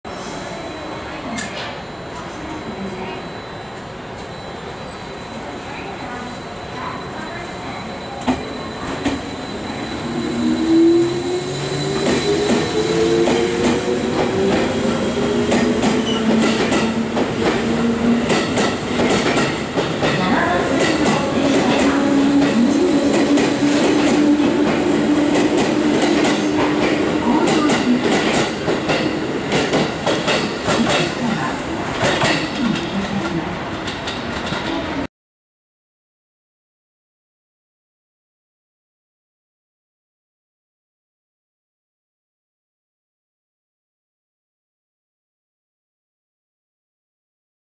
令和5年2月11日！K10編成+K9編成普通多治見行き 駅列車特集 JR中央本線 名古屋駅8番線 その298
本日は2月11日なので211系の日になりますね。まだ100両以上ある神領車両区所属の211系。2023年度は120両の315系が増備され211系が一気に廃車される可能性がありますね。